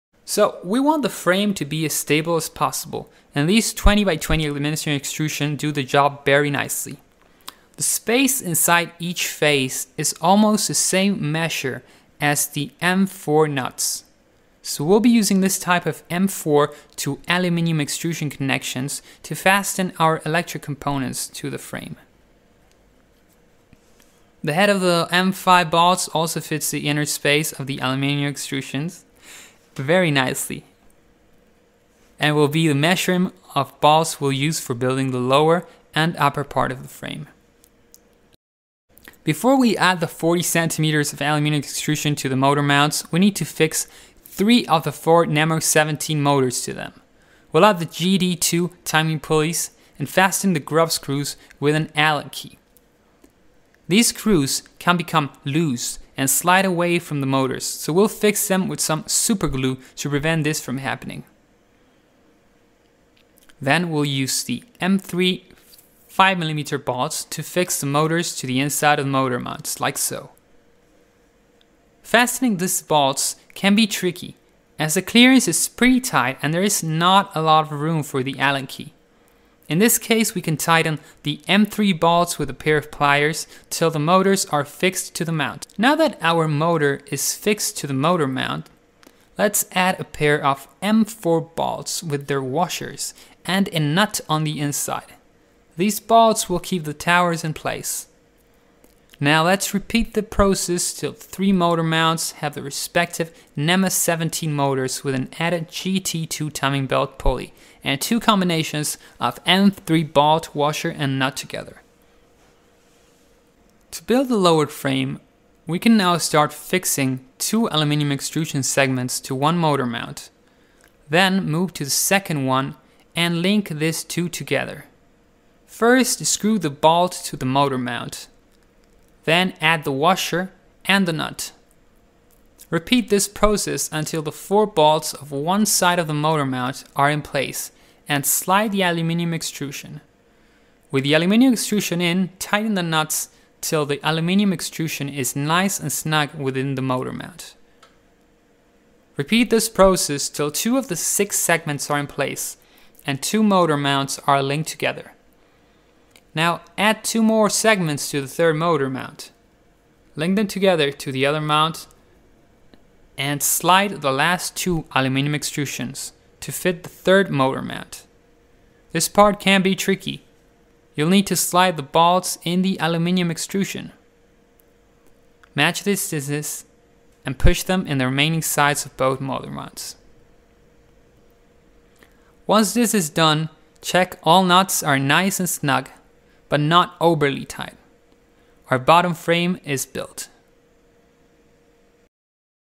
Build a Delta 3D Printer - Building the lower frame
We want the frame to be as stable as possible and these 20 by 20 aluminum extrusion do the job very nicely. The space inside each face is almost the same measure as the M4 nuts so we’ll be using this type of M4 to aluminum extrusion connections to fasten our electric components to the frame. The head of the M5 bolts also fits the inner space of the aluminum extrusions very nicely and will be the measurement of bolts we’ll use for building the lower and upper part of the frame.

Before we add the 40cm aluminum segments to the motor mounts we need to fix three of the four nema 17 motors to them. We’ll add the GT2 timing pulley and fasten the grub screws with an allen key, these screws can become loose and slide away from the motors so we’ll fix them with some superglue to prevent this from happening, then we’ll use the M3 5mm bolts to fix the motors to the inside of the motor mounts like so. Fastening the bolts can be tricky as the clearance is pretty tight and there is not a lot of room for the allen key, in this case we can tighten the M3 bolts with a pair of pliers till the motors are fixed to the motor mount. Now that our motor is fixed to the motor mounts let’s add a pair of M4 bolts with their washers and a nut on the inside, these bolts we’ll keep the towers in place. Now let’s repeat the process till the three motor mounts have their respective Nema 17 motor with an added GT2 timing pulley and two combinations of M4 bolt, washer and nut together.

We can now start fixing two aluminum extrusion segments to one motor mount, then move to the second one and link these two together. First screw the bolt to the motor mount, then add the washer and the nut. Repeat this process until the 4 bolts of one side of the motor mount are in place and slide the aluminum extrusion, with the aluminum extrusion in tighten the nuts till the aluminum extrusion is nice and snug within the motor mount, repeat this process till 2 of the 6 segments are in place and two motor mounts are linked together, now add two more segments to the third motor mount, link them to the other motor mounts and slide the last two aluminum extrusions to fit the third motor mount, this part can be tricky, you’ll need to slide the bolts in the aluminum extrusions, match the distances and push them in the remaining sides of both motor mounts. Once this is done, check all nuts are nice and snug but not overly tight, our bottom frame is built.